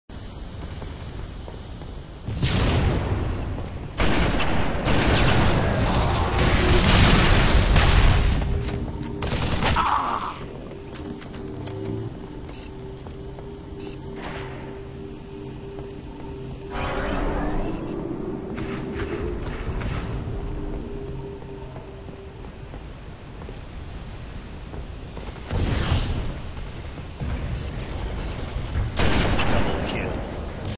2 4 1
GamerTag: Endoplasmic101 / Film Name: 2 4 1
Description: Double Kill with one sniper bullet.
Slot: 11